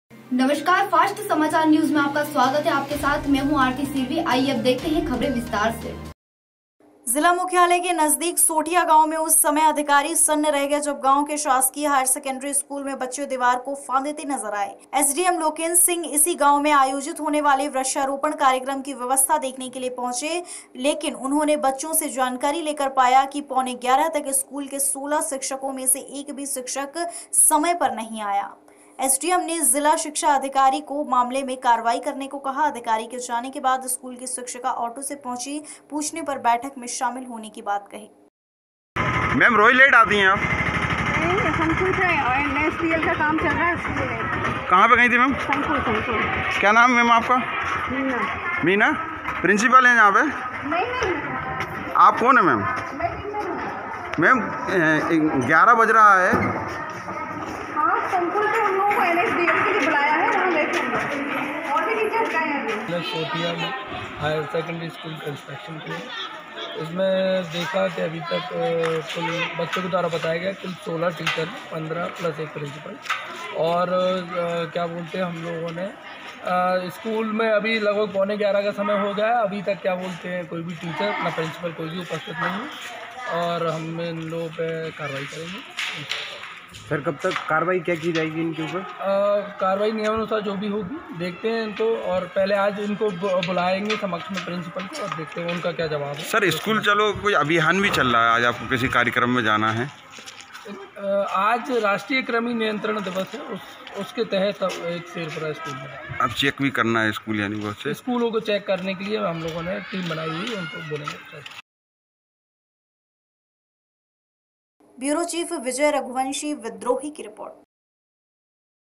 विदिशा - एसडीएम ने किया स्कूल का औचक निरीक्षण
जिला मुख्यालय के नजदीक सोठिया गांव में उस समय अधिकारी सन्न रह गए जब गांव के शासकीय हायर सेकंडरी स्कूल में बच्चे दीवार को फांदते नजर आए! एसडीएम लोकेंद्र सिंह इसी गांव में आयोजित होने बाले वृक्षारोपण कार्यक्रम की व्यवस्था देखने के लिए पहुंचे थे!उन्होंने बच्चो से जानकारी लेकर पाया कि पौने ग्यारह तक स्कूल के 16 शिक्षको में से एक भी शिक्षक समय पर यहां नही पहुचें! एसडीएम ने जिला शिक्षा अधिकारी को मामले में कार्रवाई करने को कहा!अधिकारी के जाने के बाद स्कूल की शिक्षिका आटो से पहुची, पूछने पर बैठक में शामिल होने की बात कही!